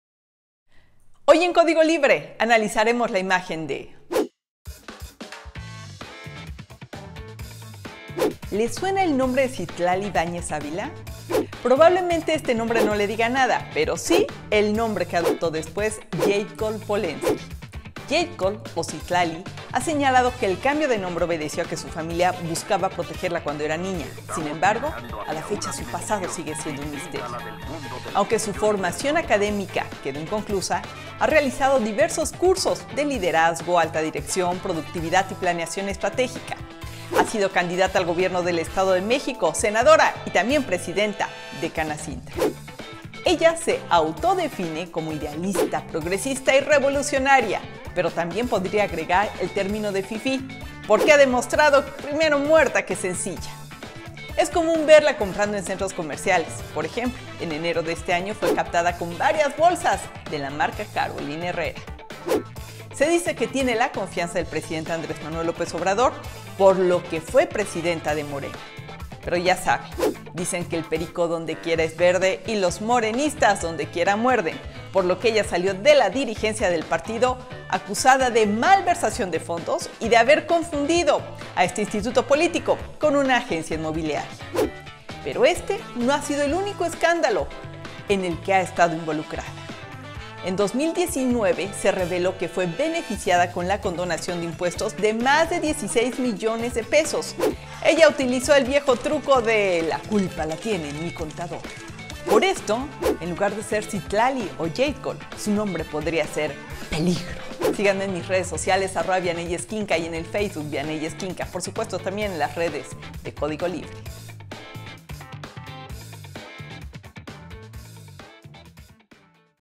¿Quién es Citlali Ibáñez Ávila alias Yeidckol Polevnsky?
En Código Libre analizamos la imagen de Citlali Ibáñez Ávila, mejor conocida como Yeidckol Polevnsky. La exidrigente nacional de Morena se autodefine como "idealista, progresista y revolucionaria".
Polevnsky dejó la dirigencia de Morena en medio de una polémica acusación de malversación de fondos. Y en 2019 se reveló que le fueron condonados impuestos por más de 16 millones de pesos.